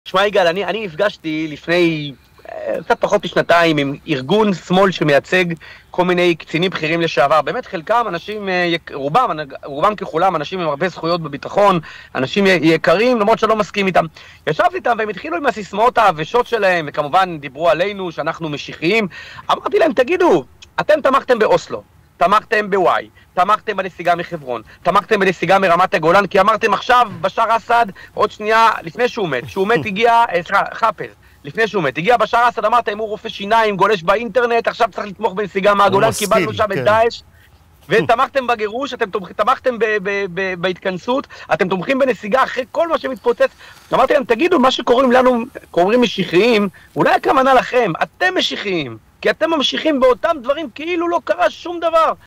האירוניה מתה! קציני צבא שתמכו באוסלו והמשיכו לתמוך בגירוש ובנסיגה מהגולן קוראים לימנים משיחיים??
בעל עסק לקבלת הצעת פרסום משתלמת:
ORI SHIFRIN‏ פלאש 90
*חדש! ערוץ היוטיוב הבינלאומי של TOV- סרטונים נבחרים של התוכן שלנו עם תרגום לשפות שונות: